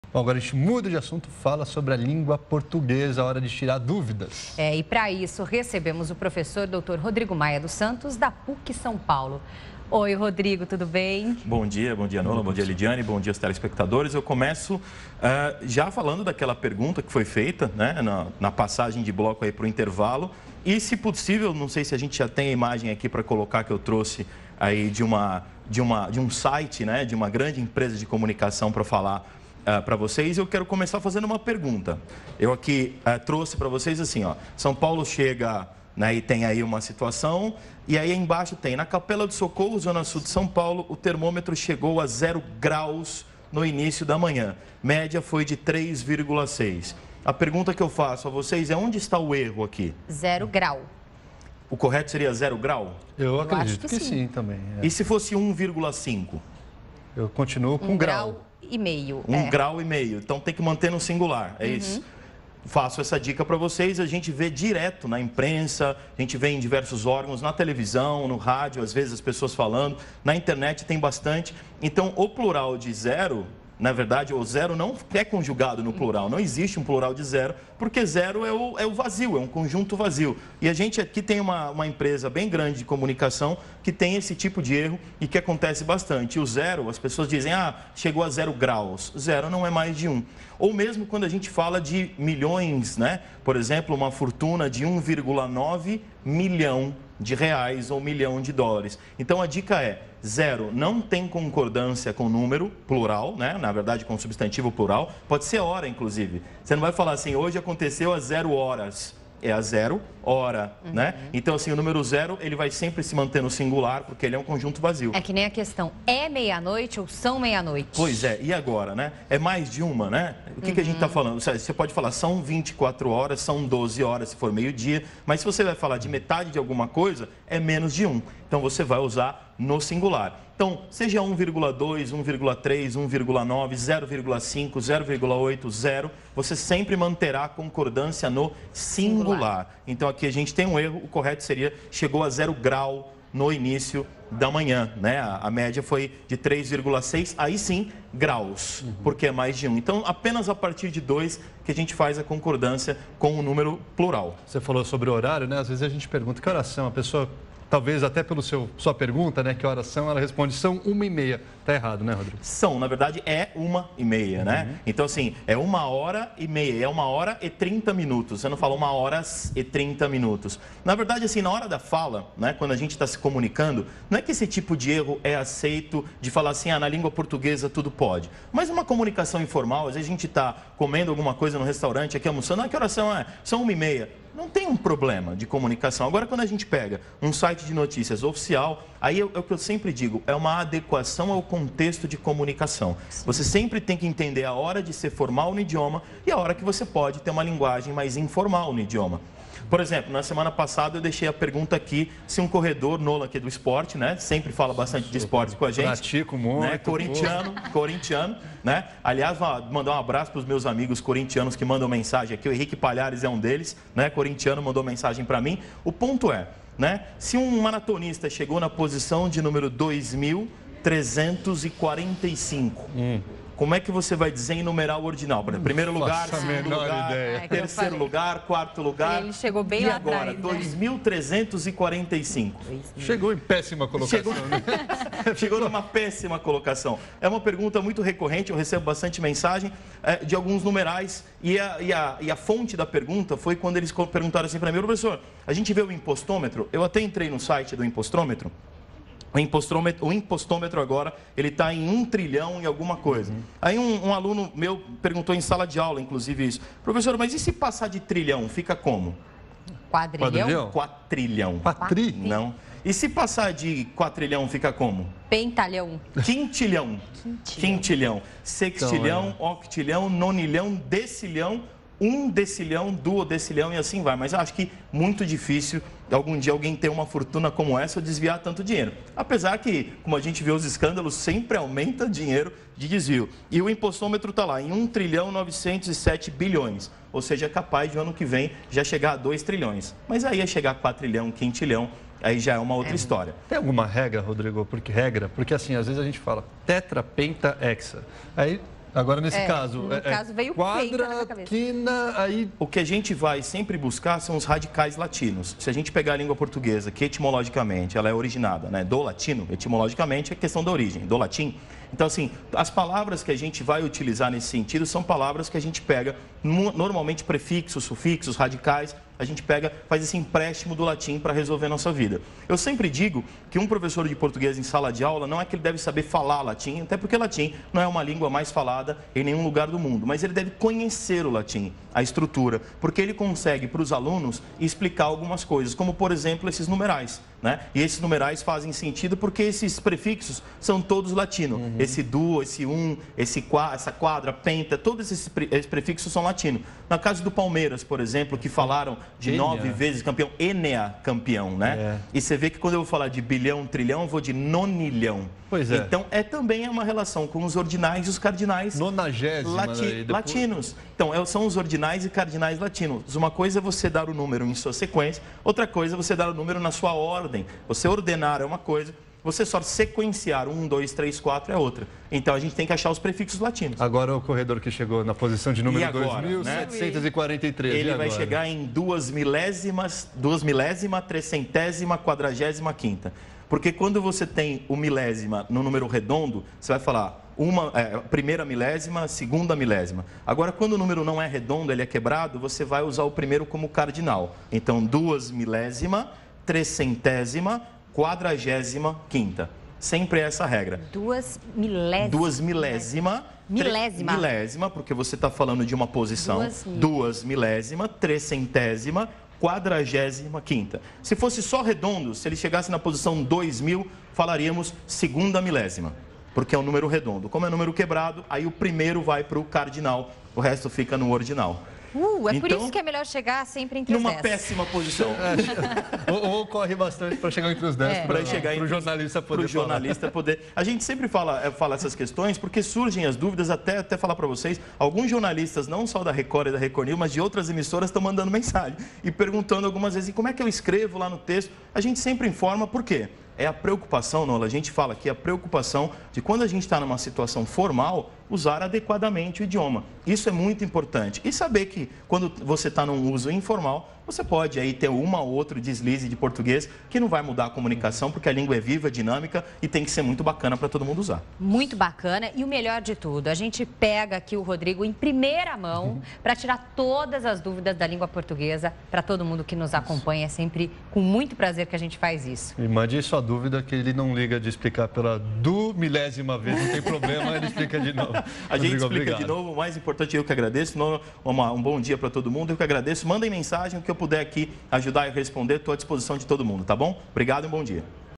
Zero grau ou zero graus? Professor Rodrigo Maia esclarece dúvidas da língua portuguesa
O professor da PUC-SP, Rodrigo Maia, respondeu as dúvidas de português dos telespectadores. Veja!